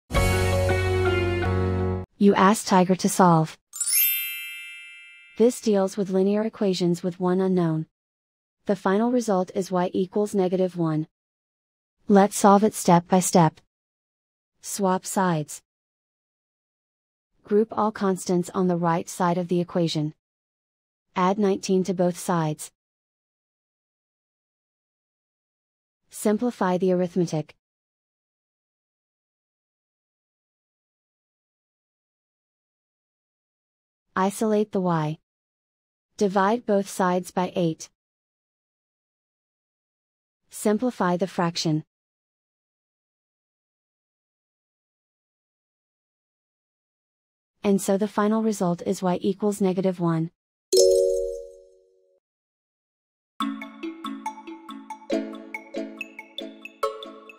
Solve -27=8y-19: Linear Equation Video Solution | Tiger Algebra
Struggling with -27=8y-19? Watch our step-by-step video on Tiger Algebra to master this linear equation effortlessly. Also check out for more tips and tricks for dealing with linear equations with one unknown